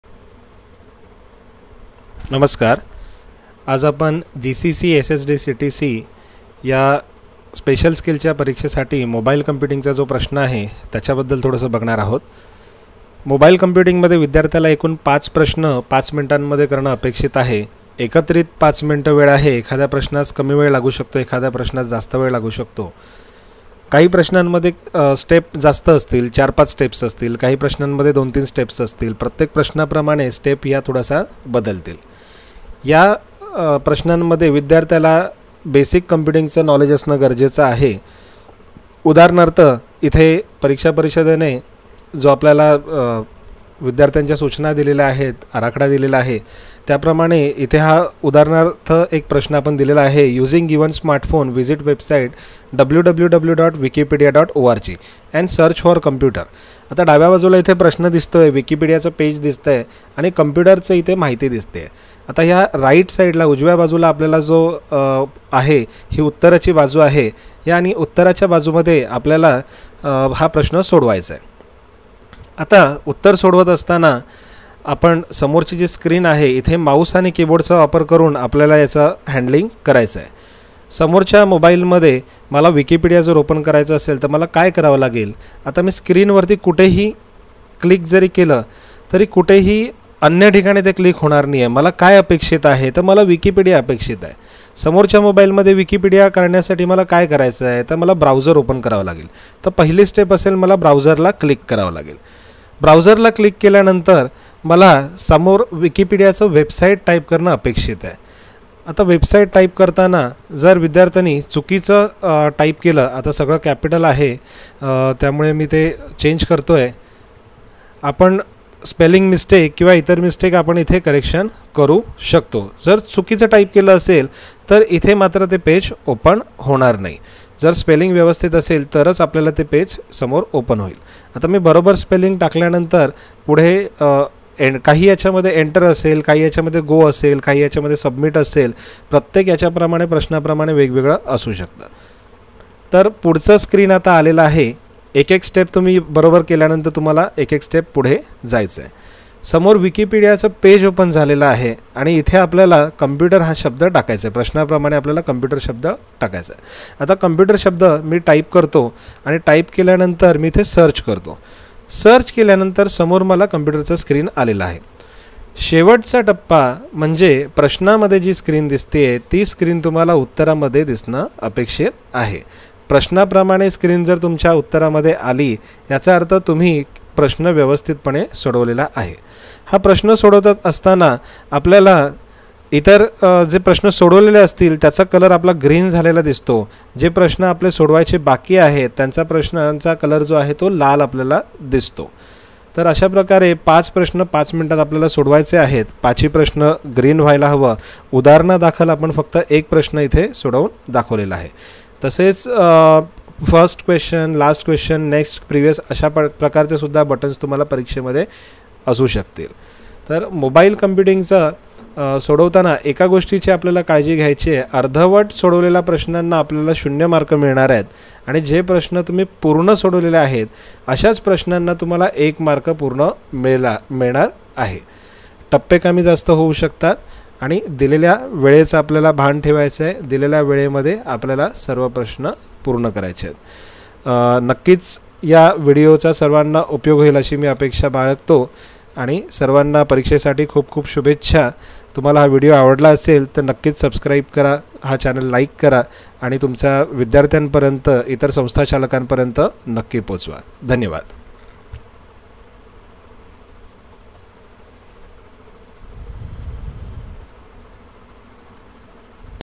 GCC TBC Special Skill (GCC SSD CTC) Mobile Computing Practice Video Tutorial
This video is made for GCC TBC computer Typing Government Authorised Institutes for Special Skill 50 wpm exam. Mobile Computing sample question is  shown with actual formatting and formulas as per  msce pune guidelines for students.